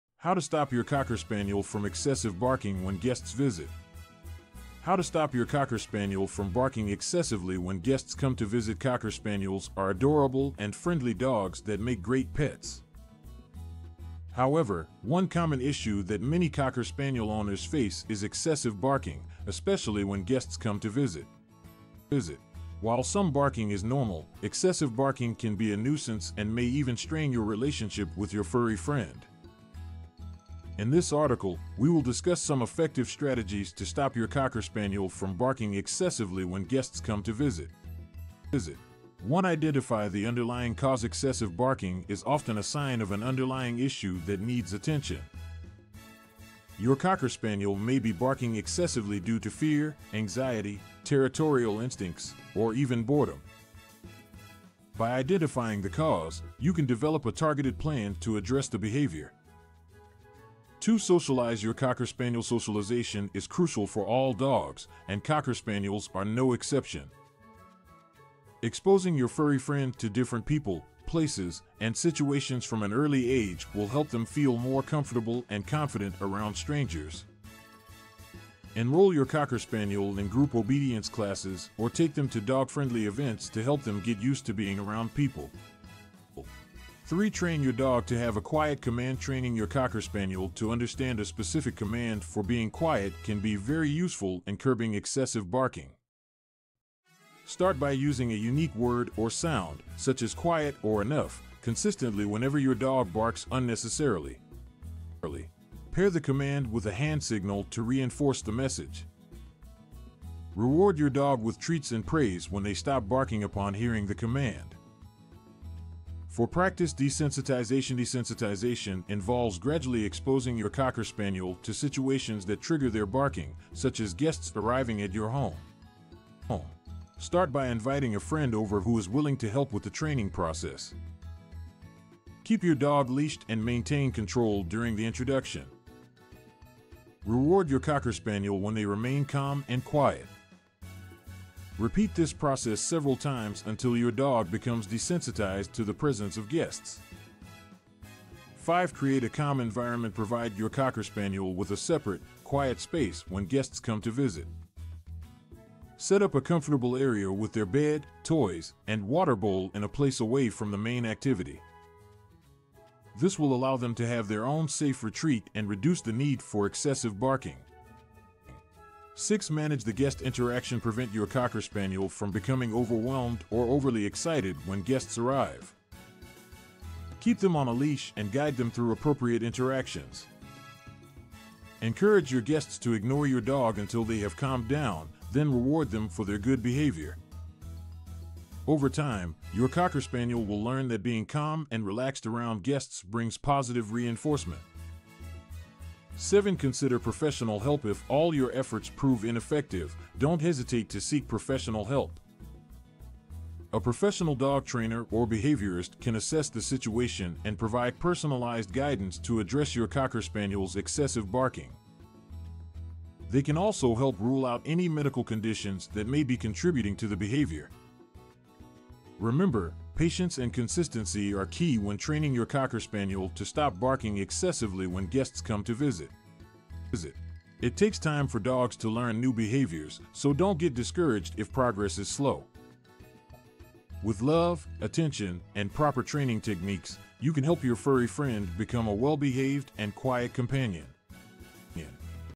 How to Stop Your Cocker Spaniel from Excessive Barking When Guests Visit
How to Stop Your Cocker Spaniel from Barking Excessively When Guests Come to Visit

Cocker Spaniels are adorable and friendly dogs that make great pets  However, one common issue that many Cocker Spaniel owners face is excessive barking, especially when guests come to visit  While some barking is normal, excessive barking can be a nuisance and may even strain your relationship with your furry friend  In this article, we will discuss some effective strategies to stop your Cocker Spaniel from barking excessively when guests come to visit

1 Identify the underlying cause
Excessive barking is often a sign of an underlying issue that needs attention
00:00 "How to Stop Your Cocker Spaniel from Excessive Barking When Guests Visit"